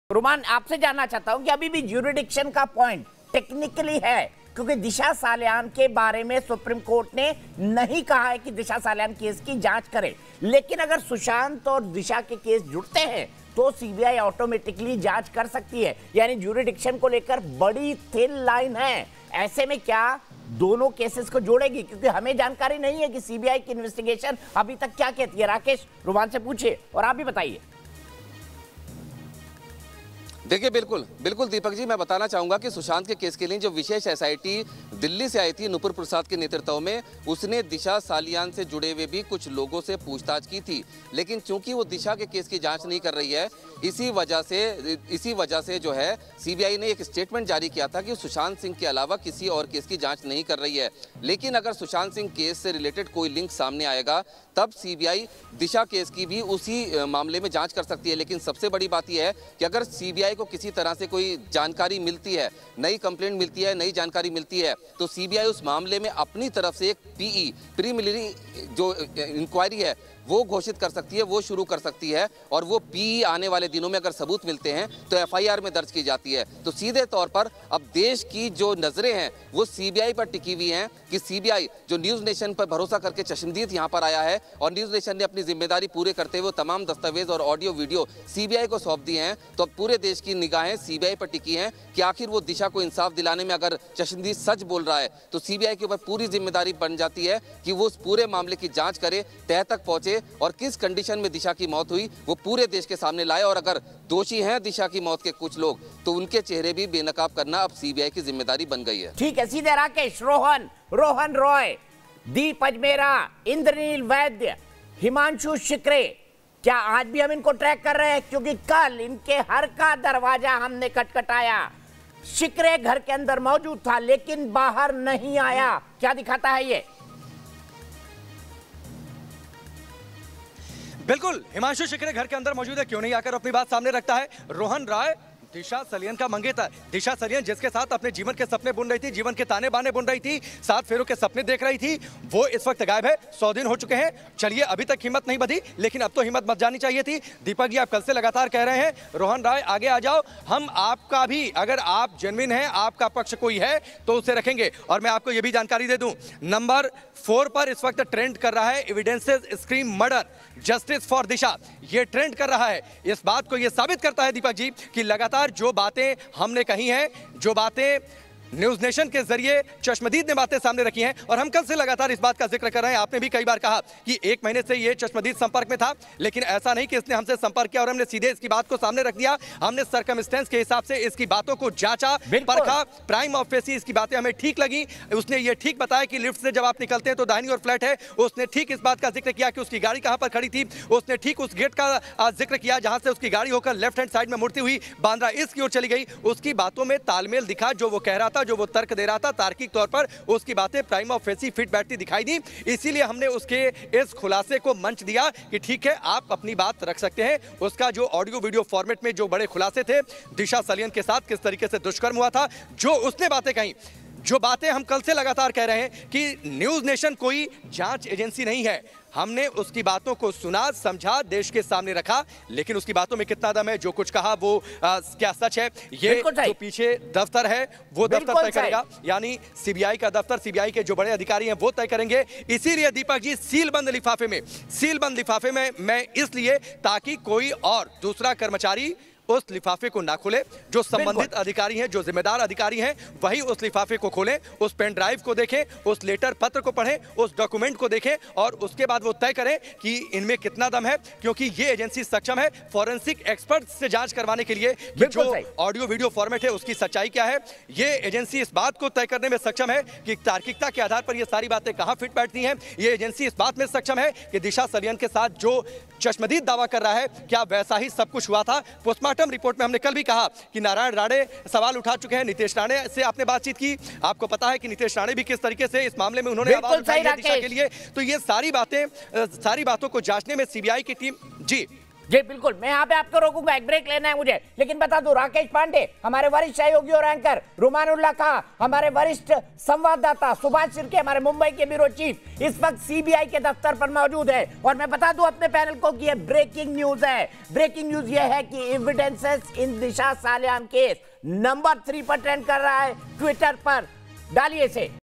Disha Salian Case : सुशांत केस से जुड़े दिशा केस तार तो CBI करेगी जांच | Big Breaking | News Nation
बॉलीवुड एक्टर सुशांत सिंह राजपूत (Sushant Singh Rajput) की मैनेजर रहीं दिशा सालियान (Disha Salian) के केस में बड़ा खुलासा हुआ है. दिशा मामले के सबसे बड़े चश्मदीद ने न्यूज नेशन (News Nation) को बताया कि पार्टी वाली रात दिशा सालियान के साथ कुल 4 लोगों ने रेप किया था. पार्टी में दिशा के मंगेतर रोहन रॉय समेत कुल 6 लोग मौजूद थे. पार्टी वाली रात दिशा की 14वीं मंजिल से गिरने से मौत हो गई थी. न्यूज नेशन के पास इस मामले में कई सबूत मौजूद हैं. शनिवार दोपहर को इन सबूतों को सीबीआई के सुपुर्द कर दिया गया.

News State Apps डाउनलोड करें:
Download Android and iOS Apps for more updates: